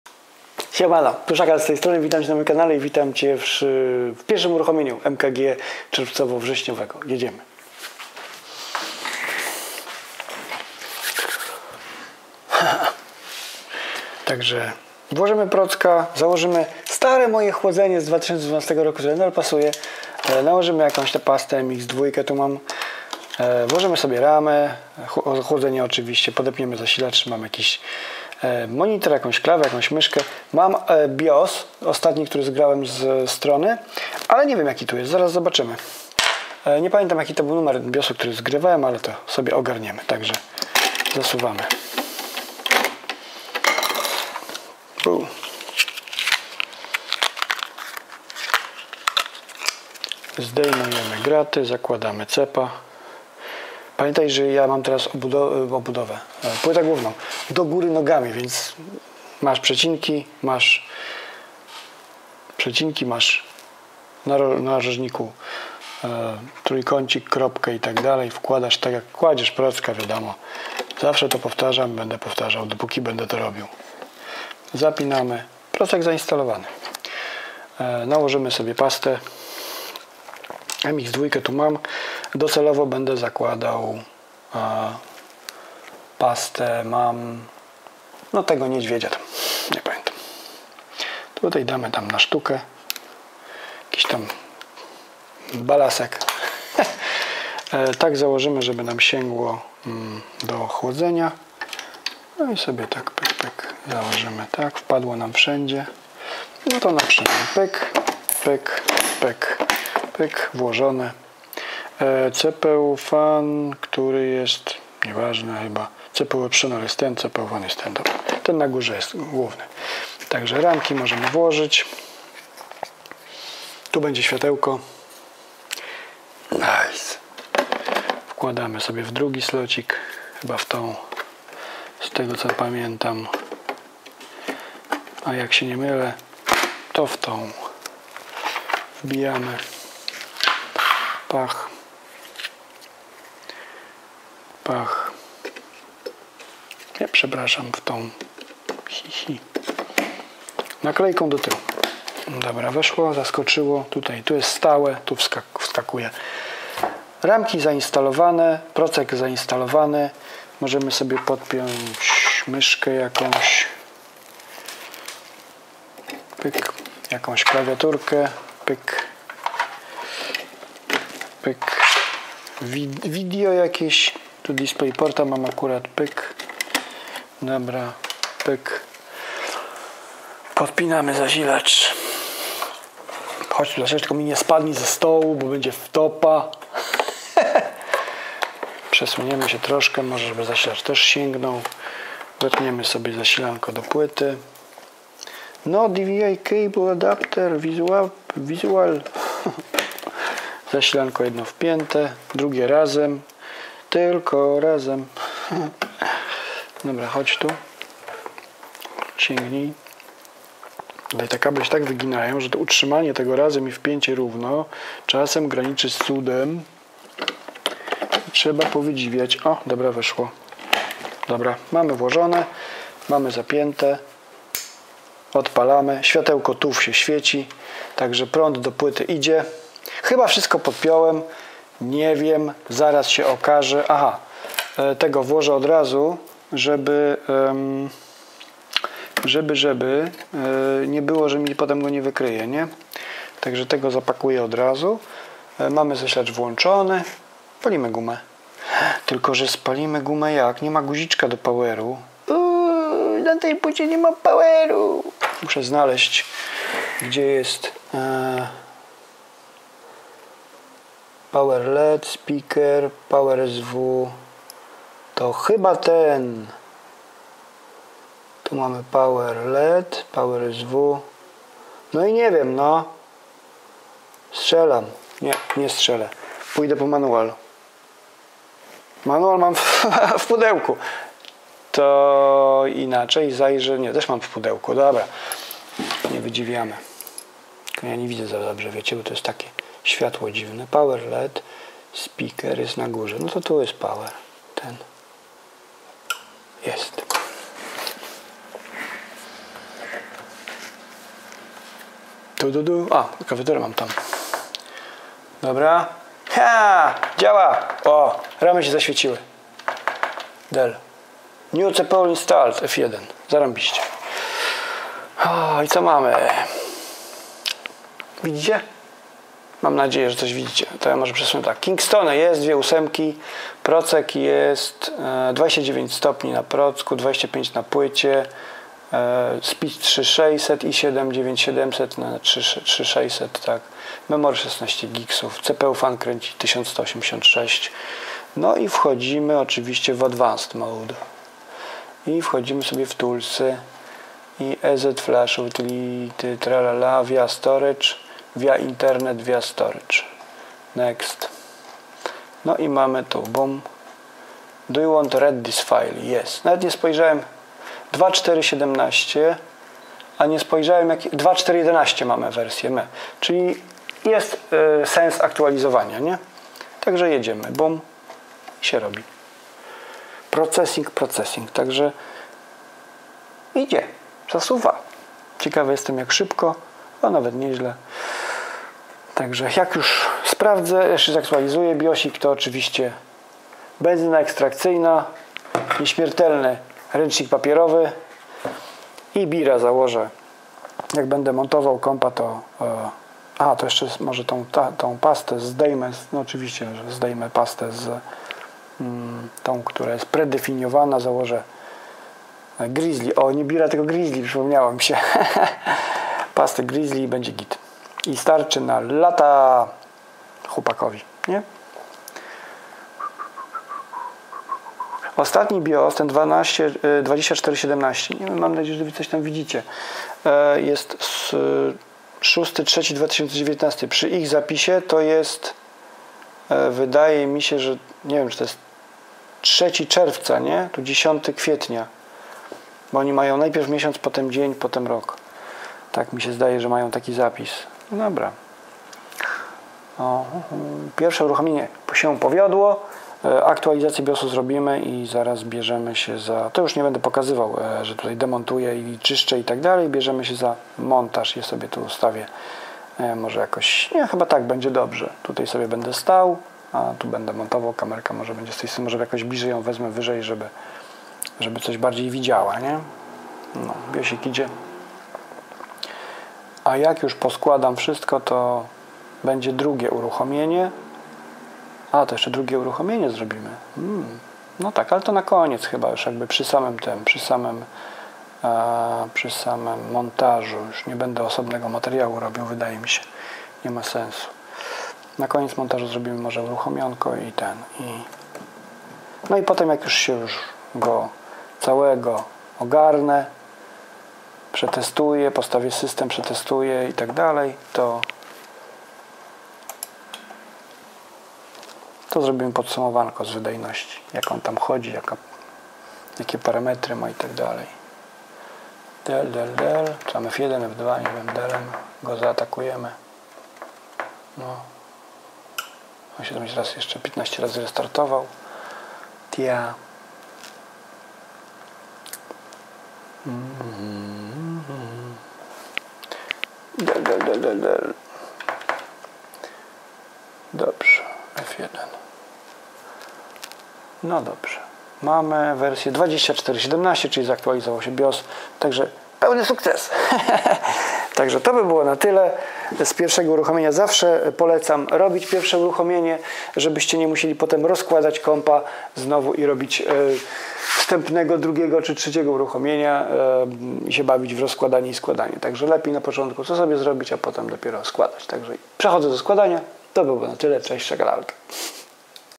MKG.i7-9700K.RTX.2080Ti.09.2019 Pierwsze Uruchomienie i Aktualizacja Biosu | 09.2019 McSzakalTV
Siema,
po Szybkich Rozpakach (Quick Unboxingach) przyszedł czas na tzw. pierwsze uruchomienie i aktualizację biosu. zobacz czy komp się odpalił i czy była w ogóle potrzeba aktualizacji biosu. Czy się udało ją zrobić?

Życząc miłego oglądania...
Pozdrawiam,
Szakal